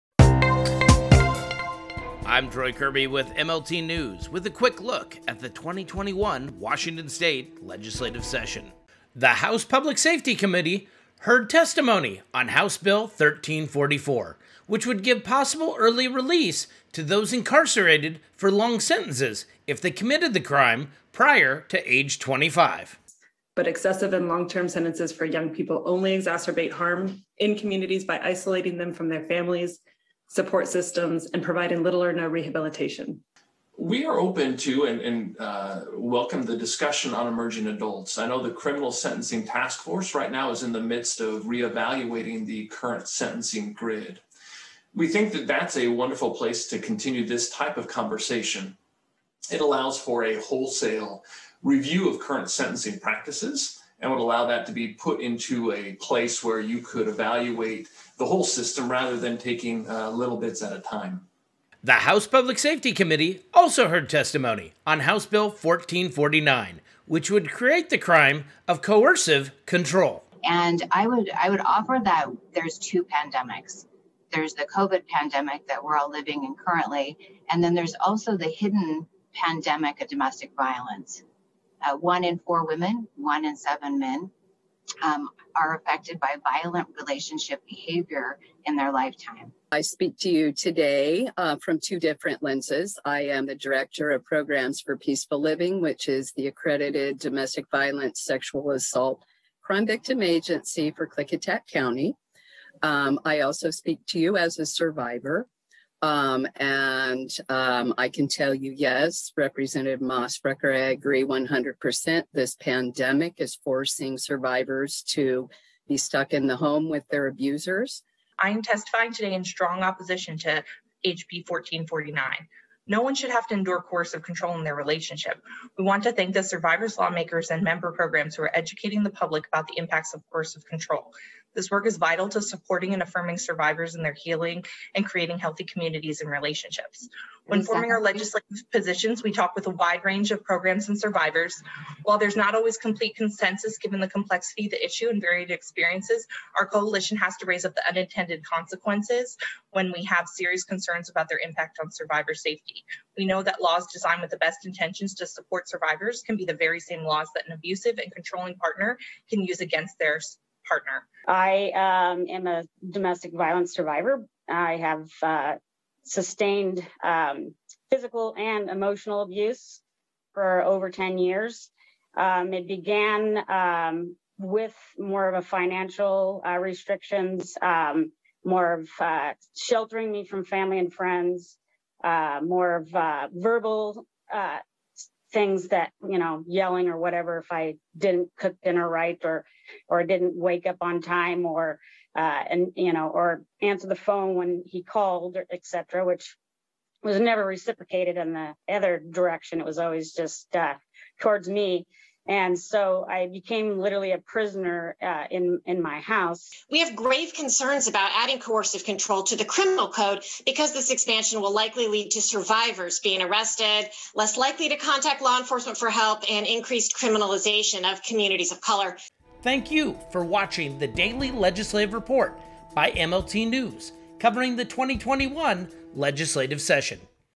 MLTnews Daily Legislative Report (Feb. 9, 2021) – Resentencing / Coercive Control
MLTnews presents The Daily Legislative Report, covering the 2021 Washington State Legislature. The House Public Safety Committee heard testimony on House Bill 1344, which would give possibly early release to those incarcerated for long sentences if they committed the crime prior to age 25. The House Public Safety Committee also heard testimony on House Bill 1449, which would create the crime of coercive control. The Housing, Human Services & Veterans committee heard public testimony on House Bill 1421, which would remove a property owner or tenant’s liability for delinquent or unpaid utility bills. The Housing, Human Services & Veterans committee also heard testimony on House Bill 1441 prohibiting discrimination against prospective tenants for unpaid rent or eviction during the COVID-19 pandemic.